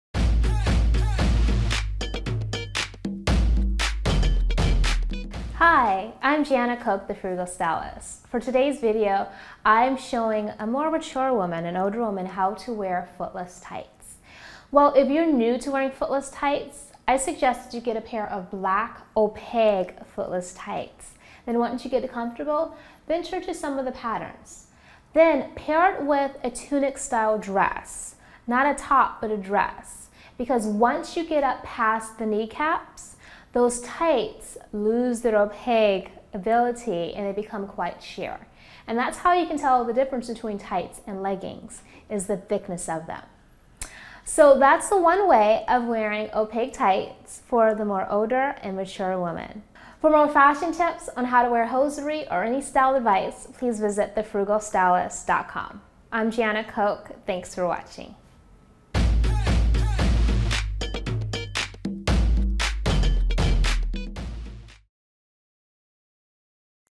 How to Wear Footless Tights for Older Women
How to Wear Footless Tights for Older Women. Part of the series: Hosiery, Stockings & Tights. Footless tights are a great accessory to wear thanks to their versatility. Wear footless tights for older women with help from a style and fashion expert in this free video clip.